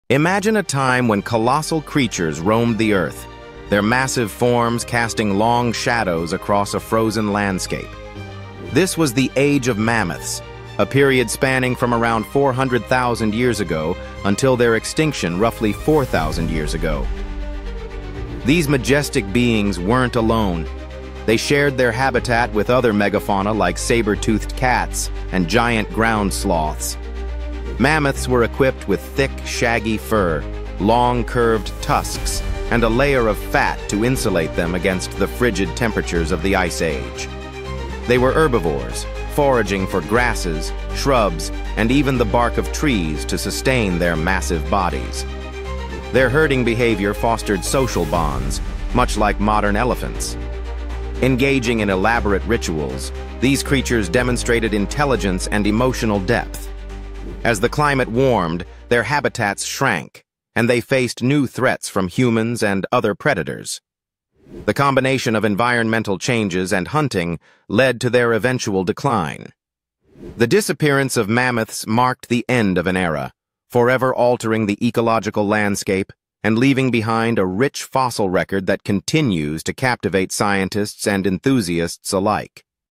Discovering the Ice Age Giants
Step into the world of the Ice Age! Explore the lives of mammoths and their megafauna companions. Uncover the mysteries that once roamed our planet.